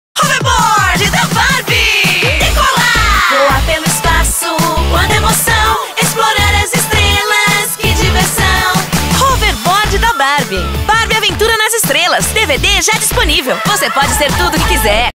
Hoverboard Da Barbie Aventura Nas Estrelas! - BR
Decole com Barbie em uma Aventura nas Estrelas com esse Hoverboard que voa de verdade! Confira como esse drone incrível funciona e se prepare para explorar a galáxia com a Barbie.